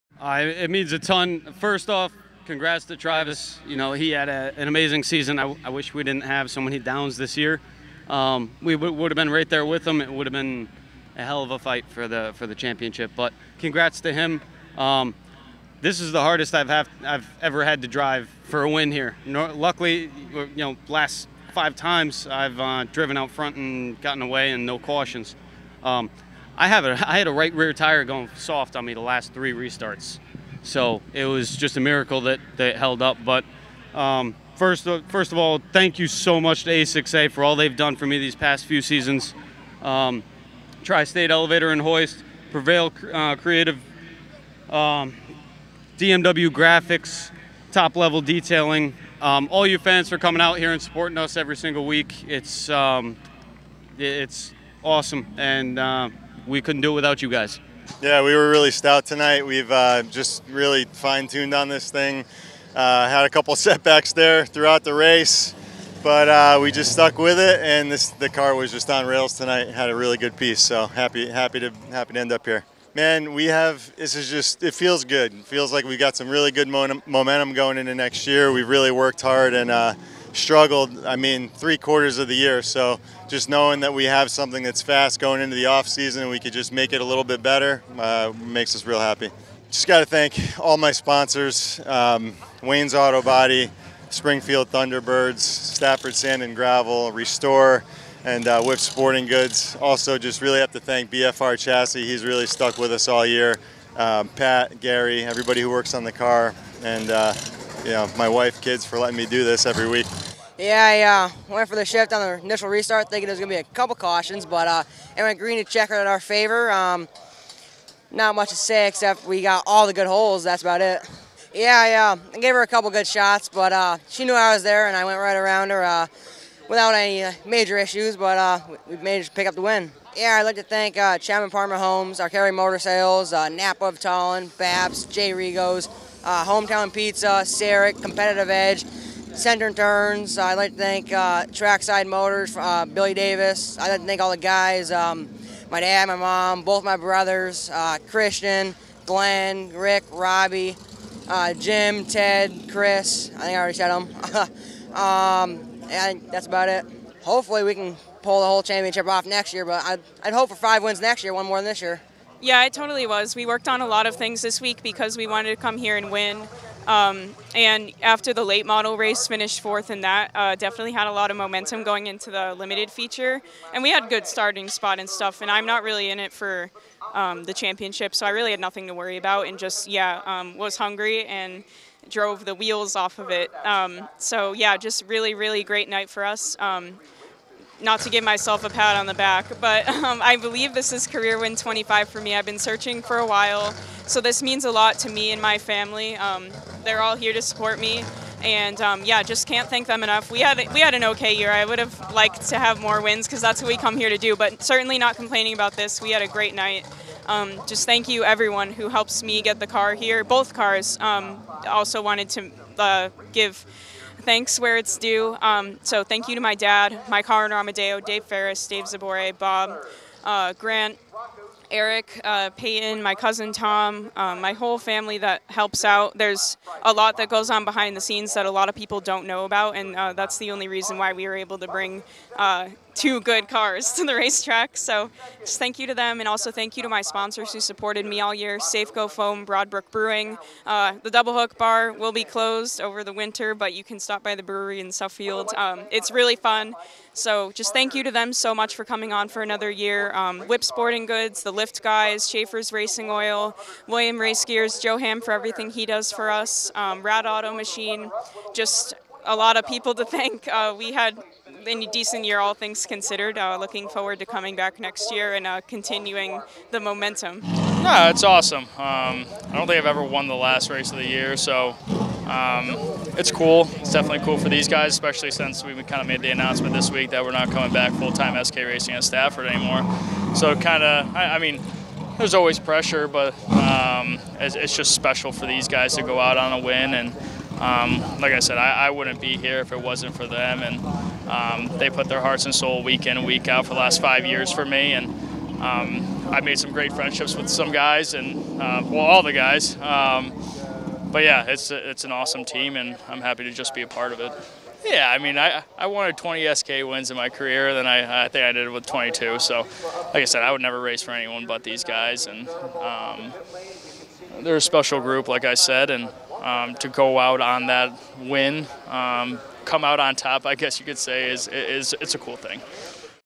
Hear from the Winners - NAPA Auto Parts Championship Night
Hear from our 5 feature winners who closed out the 2022 season with momentum in victory lane.